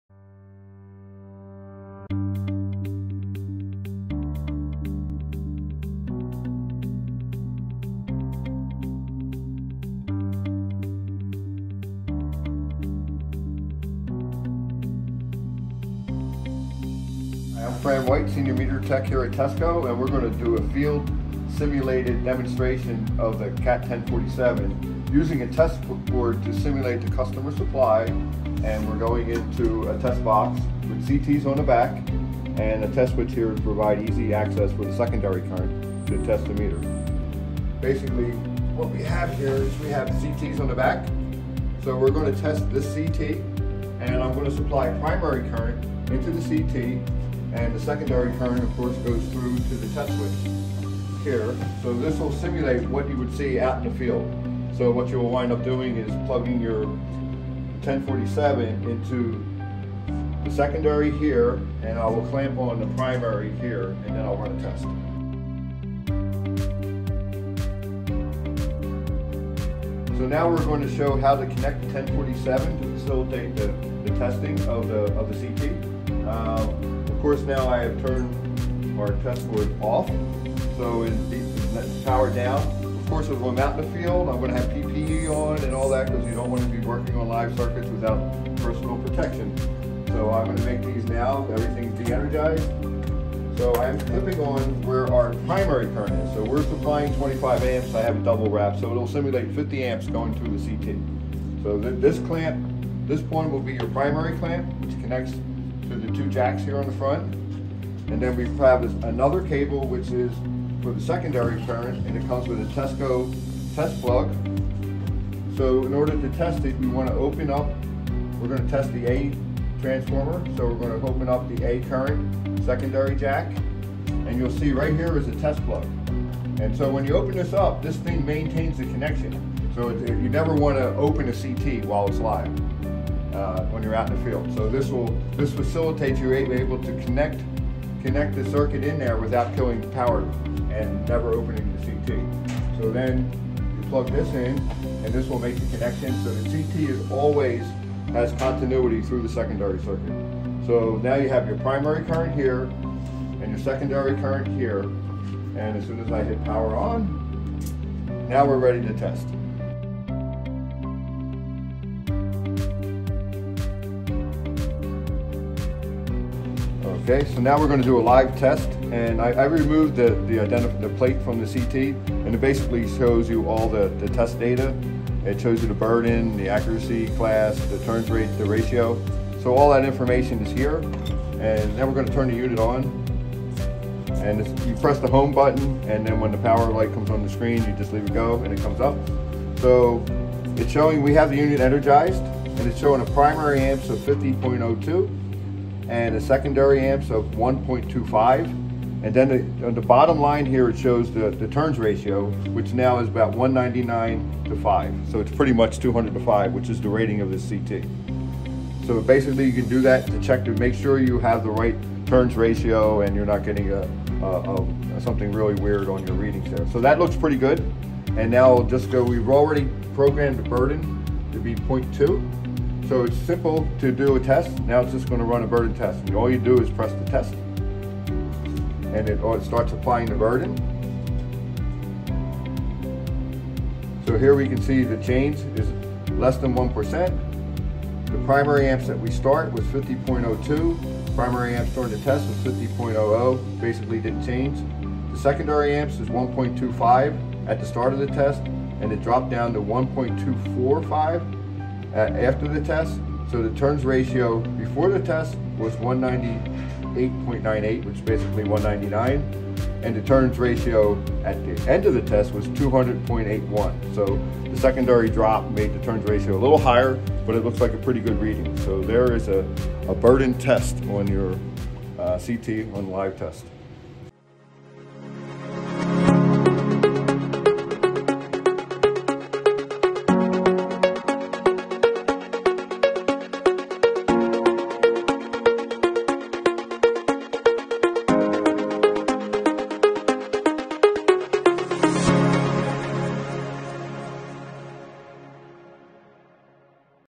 Perform a CT Burden Test with TESCO's Cat. 1047
See how to use TESCO's Cat. 1047 to perform CT Burden testing. The CT Ratio/Burden Tester can help determine if there are installation errors, loose connections, incorrect ratios, resistance buildup, open CTs, or manufacturer defects.